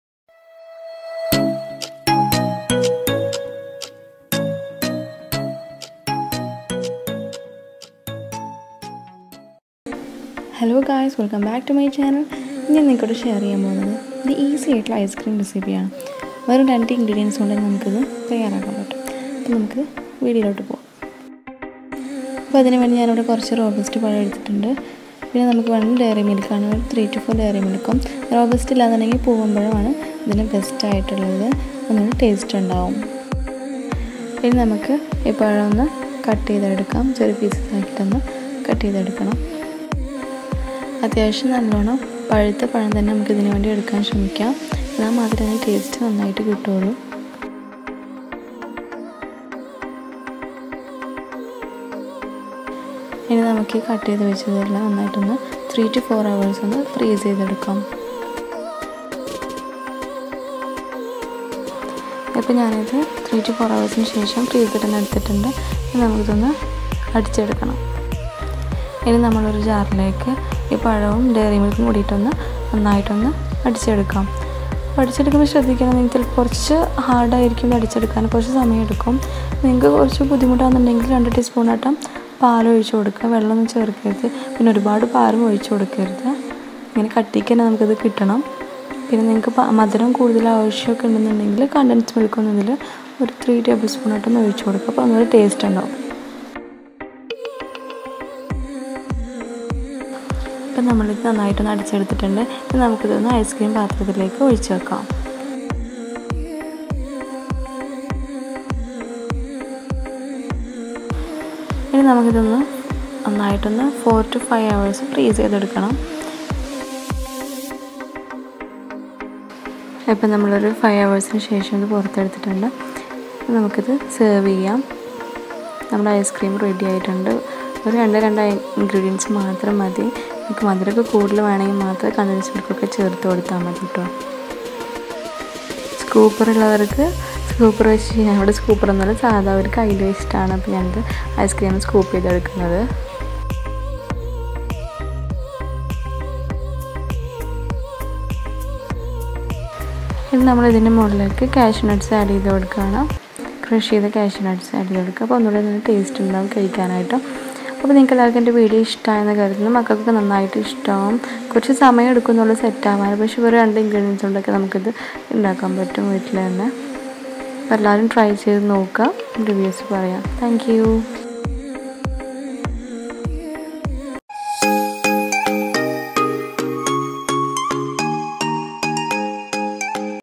EASY 2 INGREDIENTS ICECREAM RECIPE | വെറും 2 ചേരുവകൾ മാത്രം മതി | INSTANT ICECREAM RECIPE
Published on 6 sept 2020

Hy fam,

Today i show u very easy icecream recipe
Easy icecream
Instant icecream at home
2 ingredients icecream
Less ingredients icecream